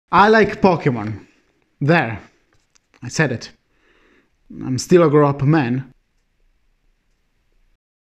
#15 Pokémon - The Seven Seconds Talk Show
Gotta Catch'em all! Merry Christmas and Happy New Birthday to my favourite franchise Pokémon for its seventh anniversary... I think.

Every Monday, Wednesday and Friday 7 pm PST.

My social:

Twitch (if this format will be liked, I’ll be happy to start a series in live on Twitch)=